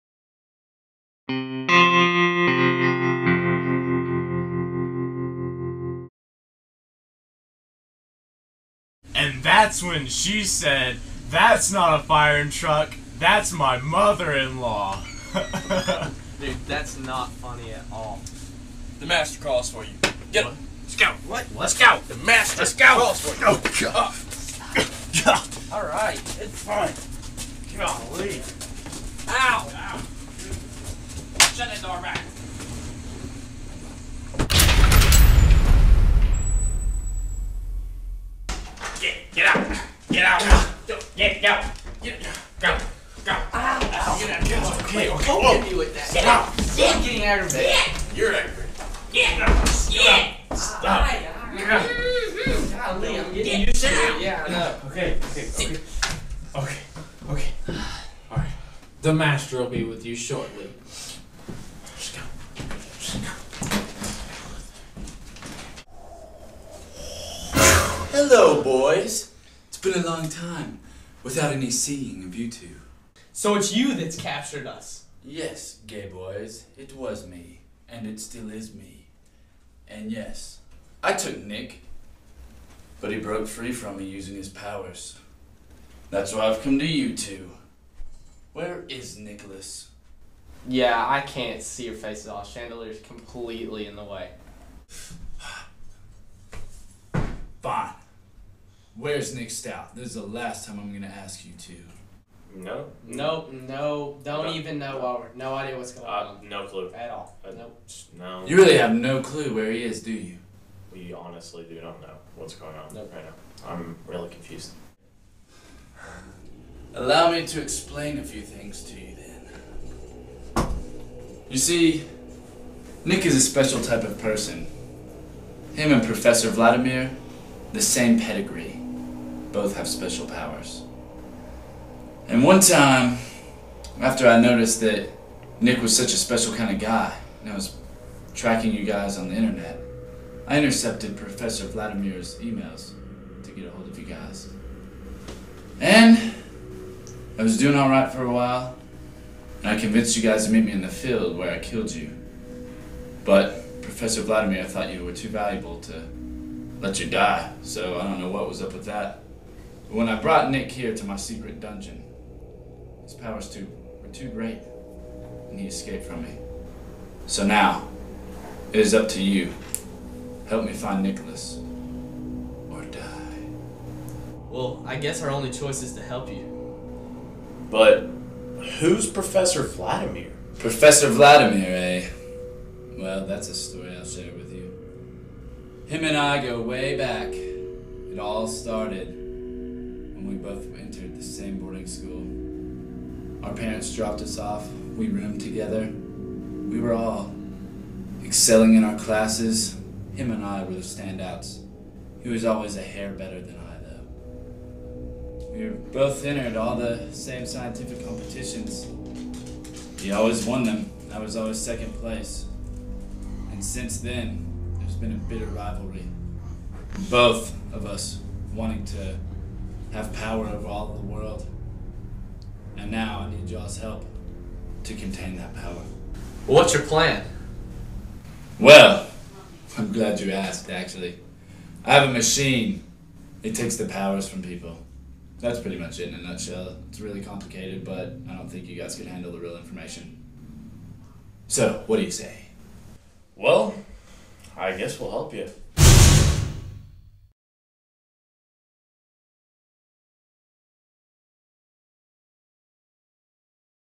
Season 2 Ep. 4 "A Hair Better"
Andrew and Adam find themselves a familiar face, and find out the real truths.